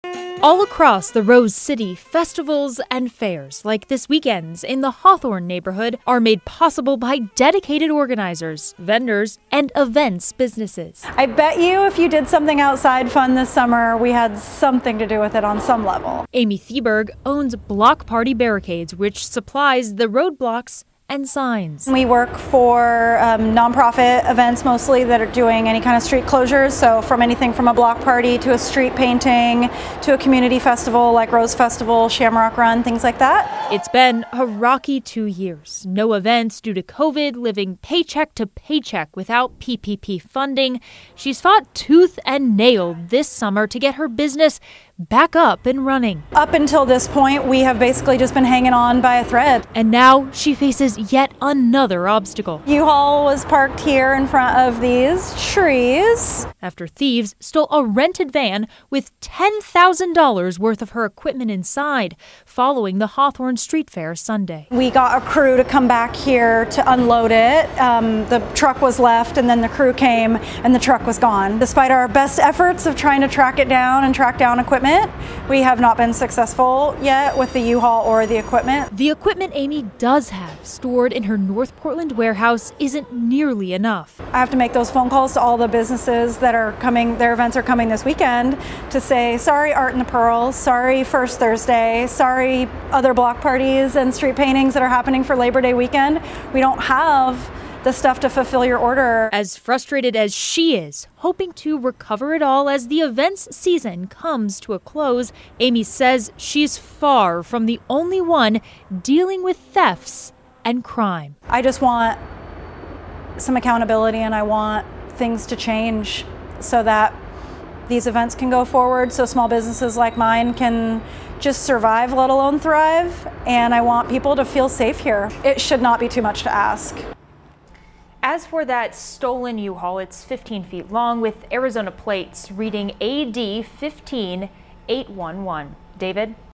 Portland business owner frustrated with theft, crime in the area and seeks accountability
A local business is suffering this week after thieves stole a rented van with $10,000 dollars' worth of equipment inside during the Hawthorne Street Fair.